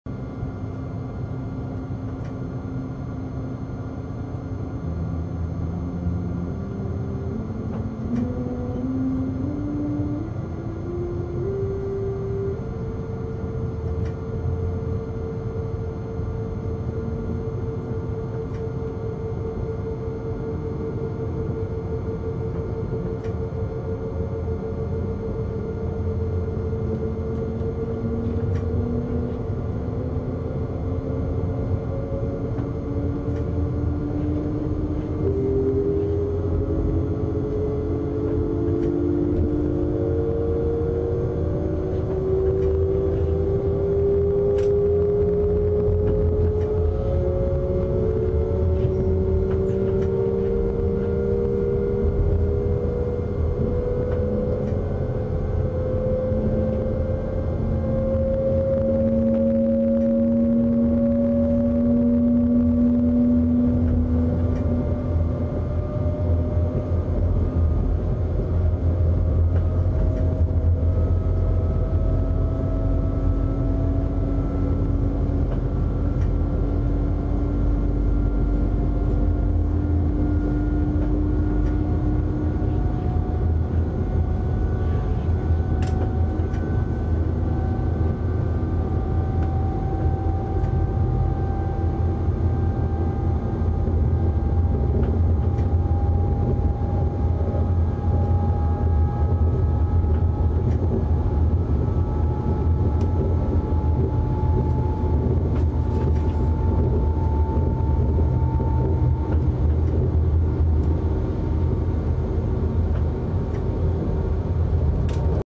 ICE anfahrt Sound
Führerstandsmitfahrt aus einem ICE 2 Berlin Spandau nach Köln bzw. Bonn.Nice Sound...